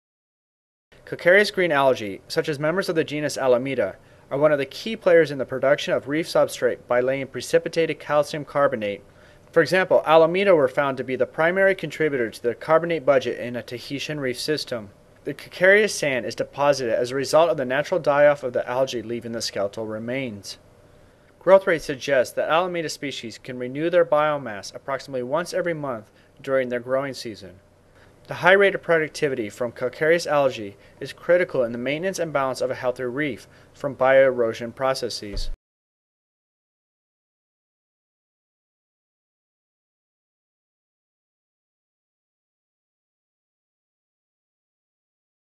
Calcareous Algae
Calcareous algae are important producers of calcium carbonate and organic matter in both tropical and subtropical marine environment (Millman 1980). Calcareous green algae, such as members of the genus Halimeda, are one of the key players in the production of reef substrate by laying precipitated calcium carbonate, i.e. limestone. For example, Halimeda spp. were found to be the primary contributor to the carbonate budget in a Tahitian reef system (Payri 1988). Growth rates suggest that Halimeda spp. can renew their biomass approximately once every month during their growing season (Wefer 1980). The calcareous sand is deposited as a result of the natural die-off of the algae leaving the skeletal remains and the consumption by some herbivores such as parrotfish. The high rate of productivity from calcareous algae is critical in the maintenance and balance of a healthy reef from bio-erosion processes.

Credits
Cinematography: Dr. Stuart Sandin
Edited by: Neilan Kuntz
Written by: Dr. Olga Pantos
Location: Palmyra Island, Line Islands, Central Pacific (2004)

Milliman JD. (1974) Marine Carbonates. Springer, Berlin Heidelberg New York, p 375.

Payri, CE (1988) Halimeda contribution to organic and inorganic production in a Tahitian reef system. Coral Reefs 6:251-262.

Wefer G (1980) Carbonate production by algae Halimeda, Penicillus and Padina. Nature 285:323-324.